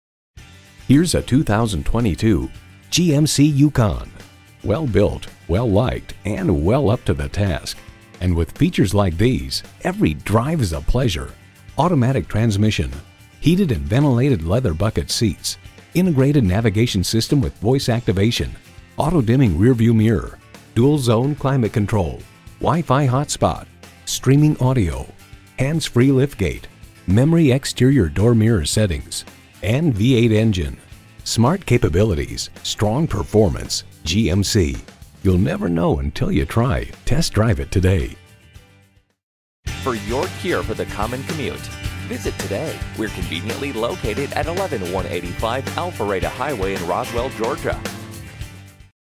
Used 2022 GMC Yukon Roswell GA Atlanta, GA 10595P - SOLD
Year: 2022
Make: GMC
Model: Yukon
Trim: Denali
Engine: EcoTec3 6.2L V8
Transmission: 10-Speed Automatic with Overdrive
Color: Black
Interior: Jet Black
Mileage: 13234
Stock #: 10595P
VIN: 1GKS1DKL2NR353321

Address:
11185 Alpharetta Hwy